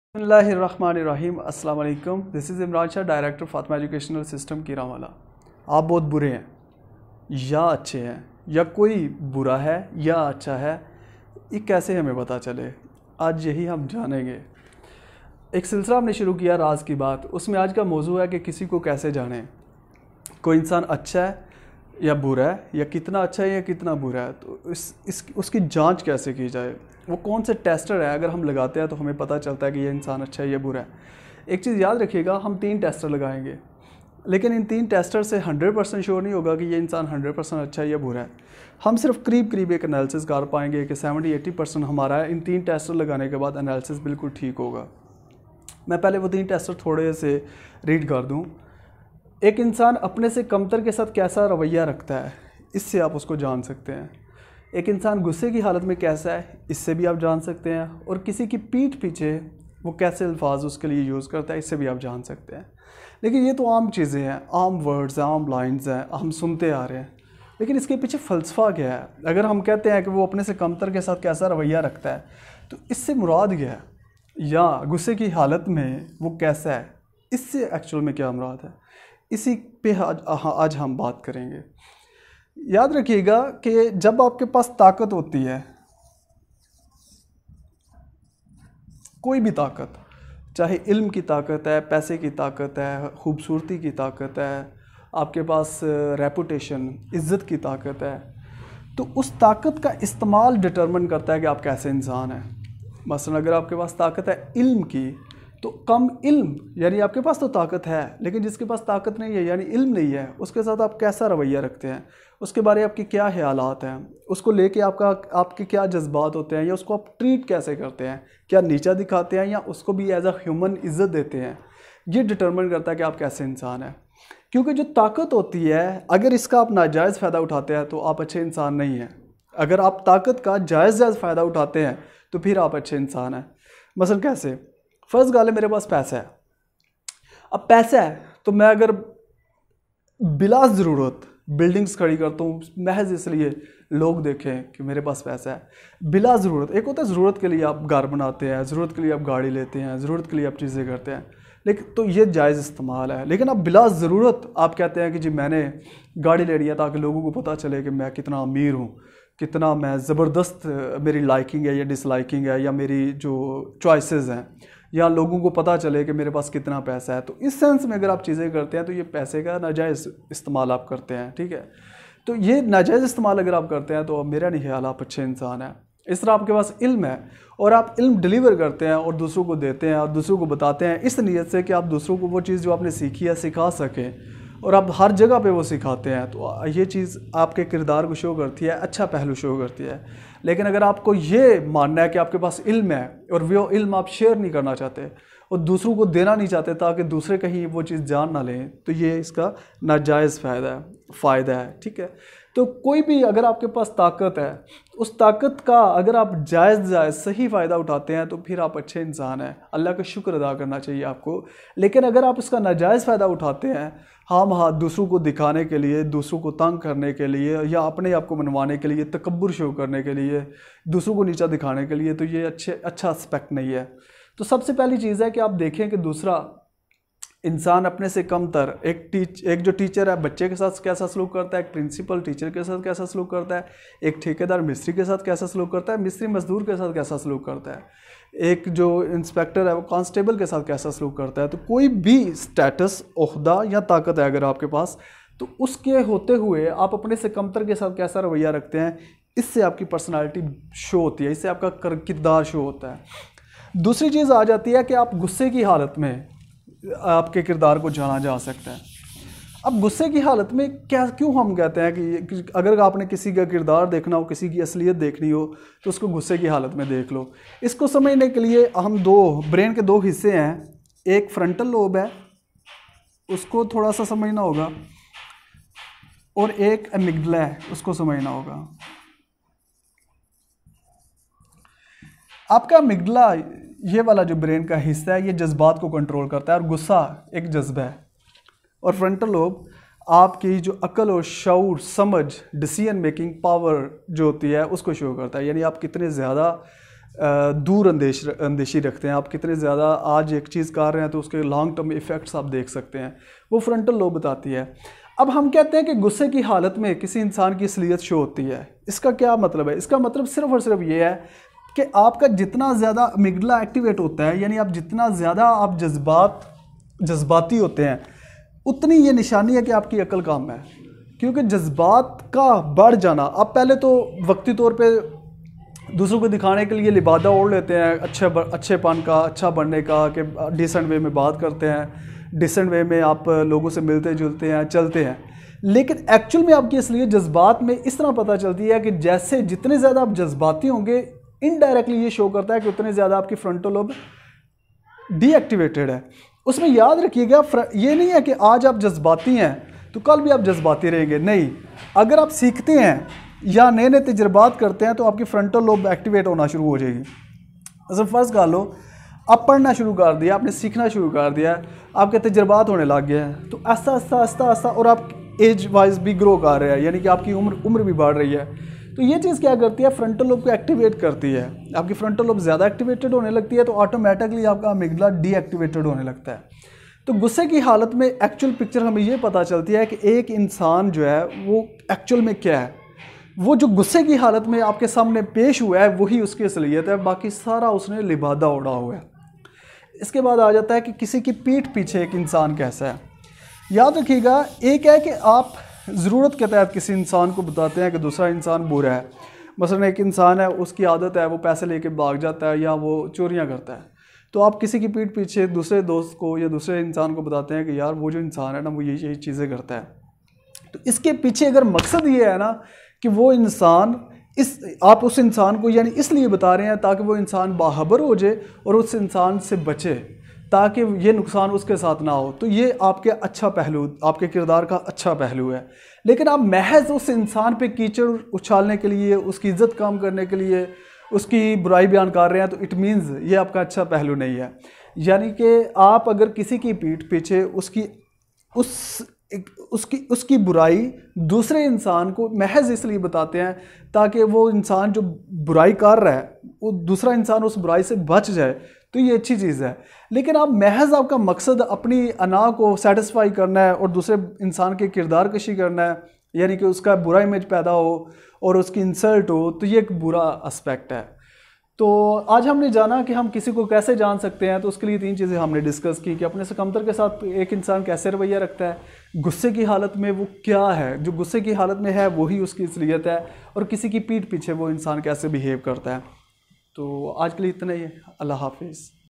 سلسلہ : راز کی بات موضوع : کسی کو کیسے جانیں کے وہ کیسا ہے ۔
how to know others ? by Imran Shah